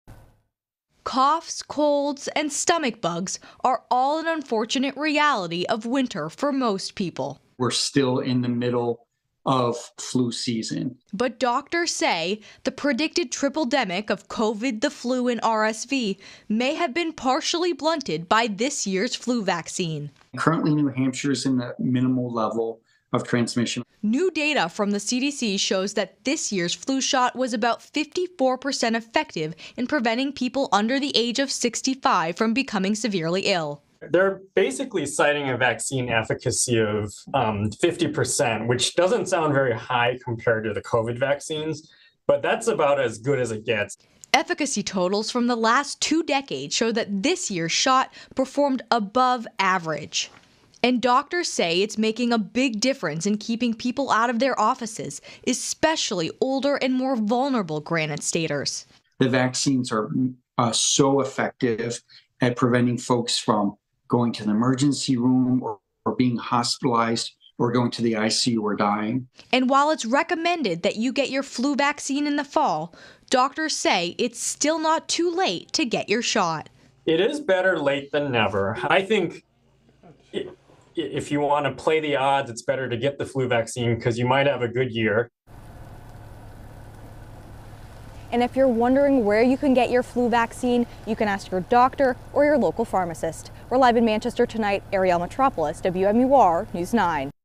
CDC data: Flu shots have been working well this season
New data from the Centers for Disease Control and Prevention (CDC) shows that the flu vaccine is working well in protecting people against severe disease, this winter.